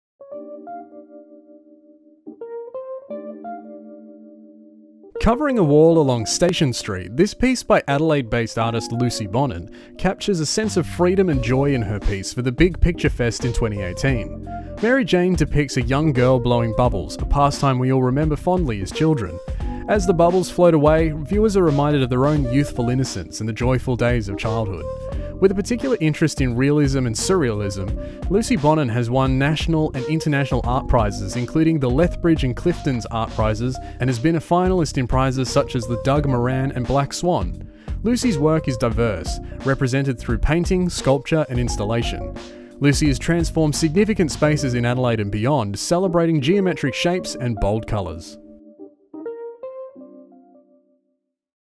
Frankston City - Digital Street Art Walking Tour - Mary Jayne - Lucy Bonnin
Mary-Jayne is artwork that conjures memories of childhood play and wonder. The whimsical bubbles and plaited hair float across the wall like a memory that floats across your thoughts.

Artist Bio:
Lucy’s style and work is diverse, usually focusing on painting tonal realism with a signature touch of surrealism. Based in Adelaide, South Australia, she studied Fine Art at Adelaide Central School of Art. She has won local, national and international art prizes with works being shown across Australia in public and private settings.

This video forms part of Frankston City's Digital Street Art Walking Tour. Download the Discover Frankston mobile app from App Store or Google Play Store to take the tour and discover more artwork on the trail.